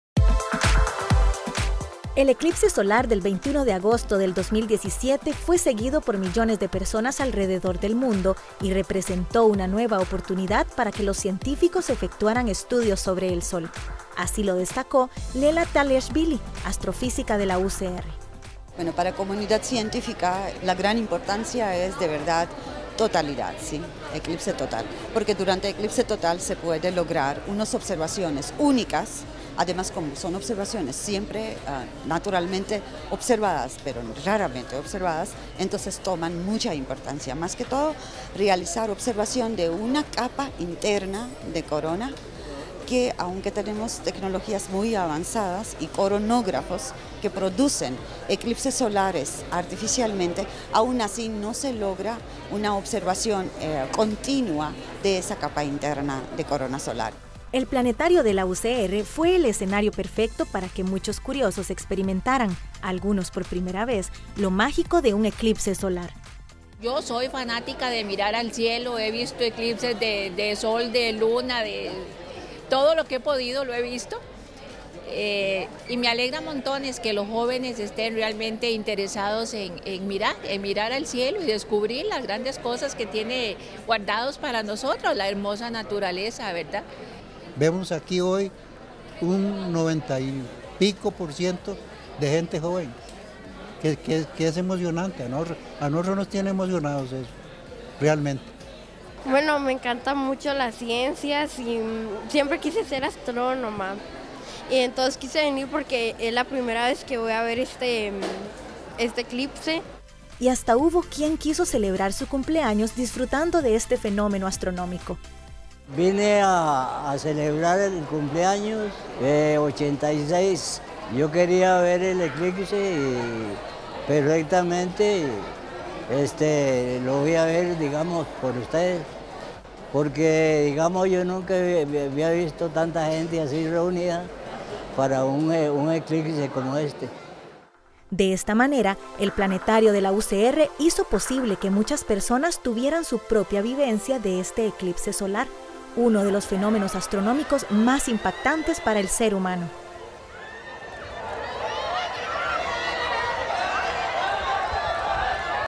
Eclipse desde el Planetario UCR
El Planetario de la UCR, único en Centroamérica, abrió sus puertas y dispuso sus herramientas al servicio de niños, jóvenes, adultos mayores y medios de comunicación, quienes durante tres horas pudieron seguir de cerca el desarrollo del Eclipse Solar en nuestro país y alrededor del mundo.